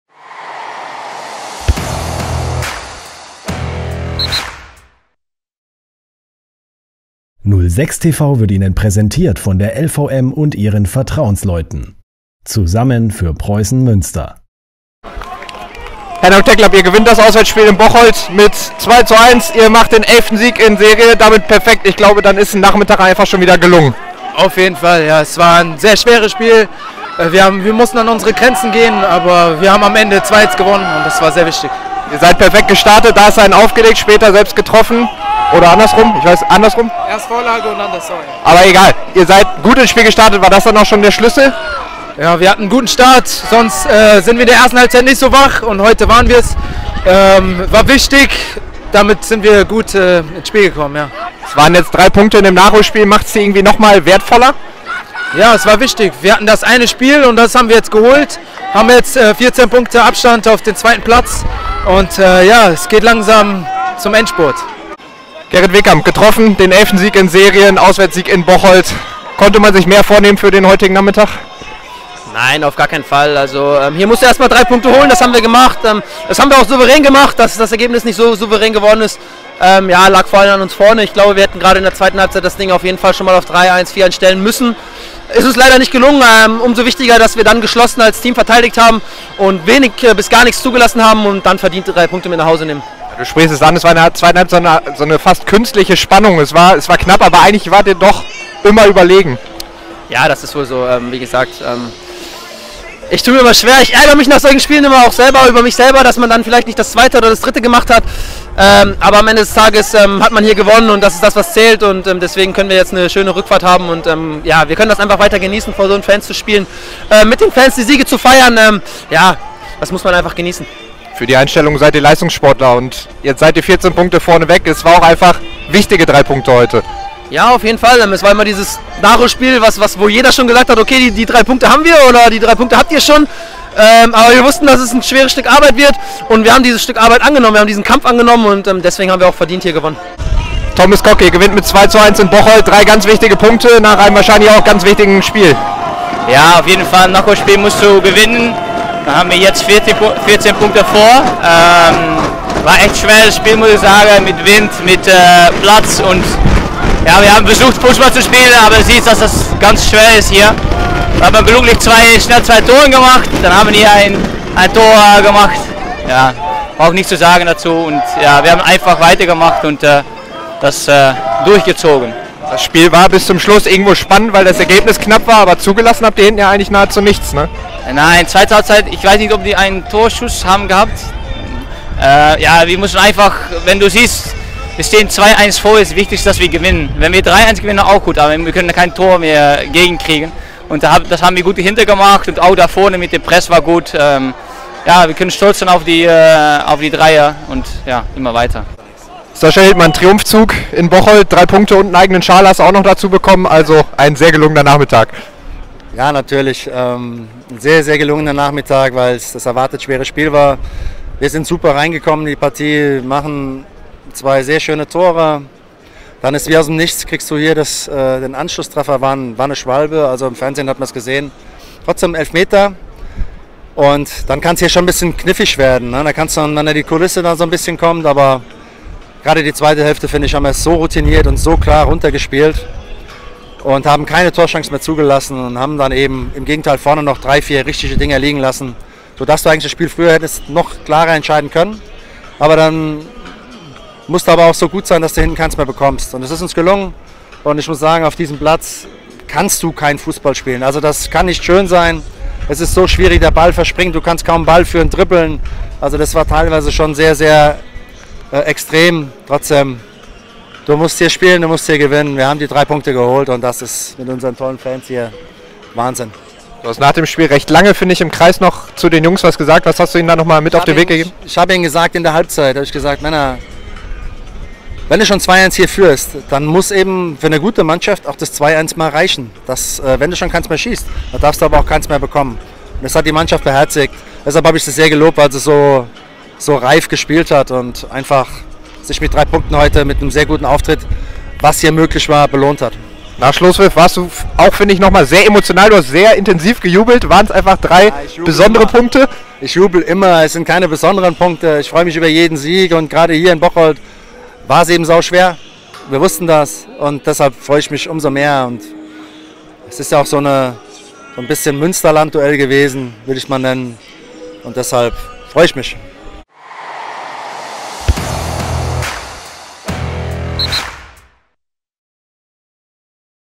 20. Spieltag: FC Bocholt - SC Preußen Münster (O-Töne)
Es war ein hartes Stück Arbeit, das die Adlerträger am Hünting erwartete. Doch die Preußen nahmen den Kampf unter schwierigen Bedingungen an und setzten sich am Ende auch verdient gegen den FC Bocholt durch. Das sahen auch Henok Teklab, Gerrit Wegkamp, Thomas Kok und Trainer Sascha Hildmann im Anschluss an die Partie so. Zu sehen und zu hören gibt es die O-Töne natürlich bei nullsechs.TV.